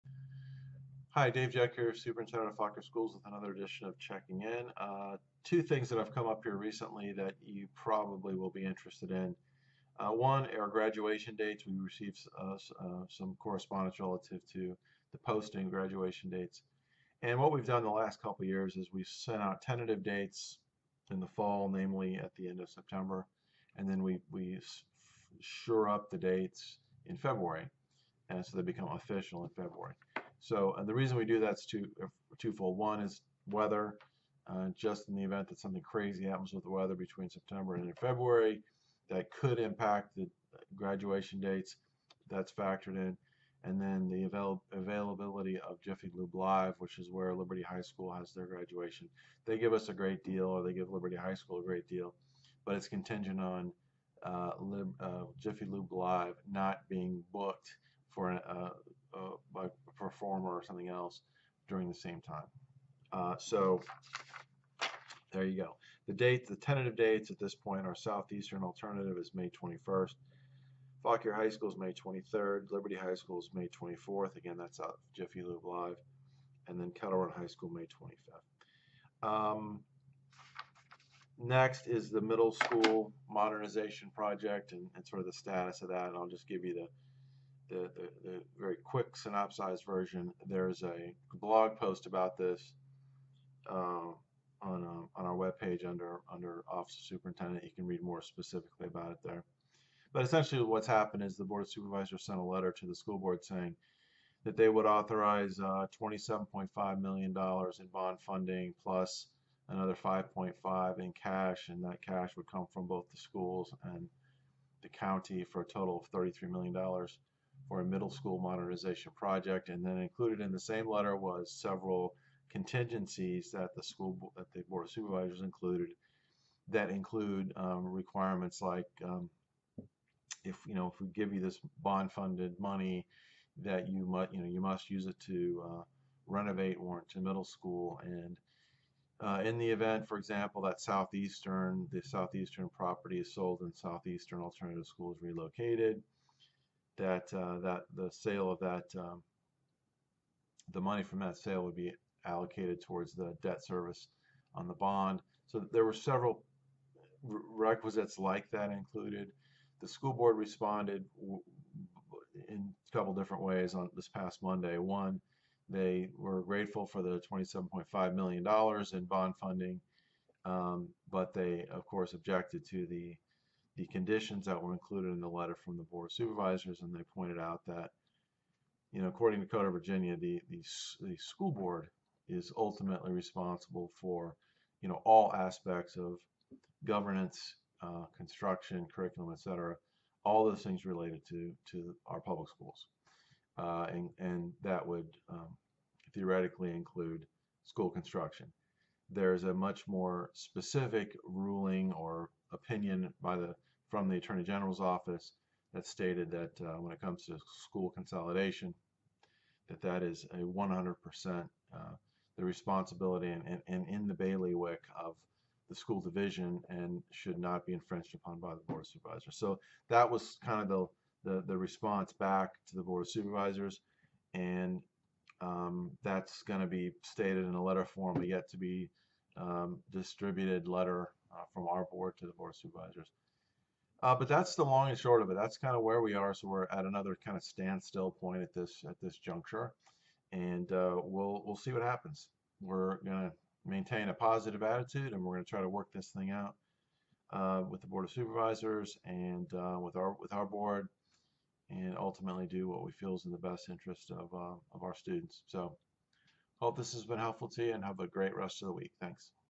CIDJ 17-09-21
Two things that have come up here recently that you will probably be of  interested. One are graduation dates, and the second is  the Middle School Modernization Project, and the status of it.